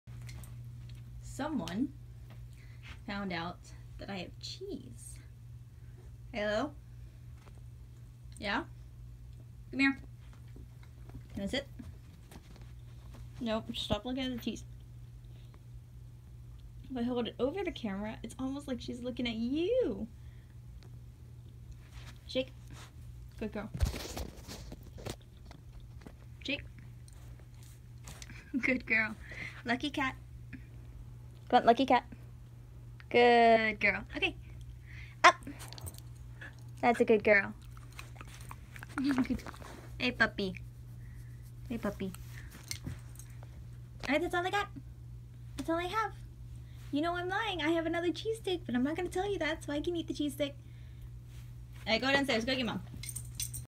One (1) piece of cheese
my tummy hurt so i didn’t want to rush edit today’s cake pop and get it out before midnight. this will have to suffice.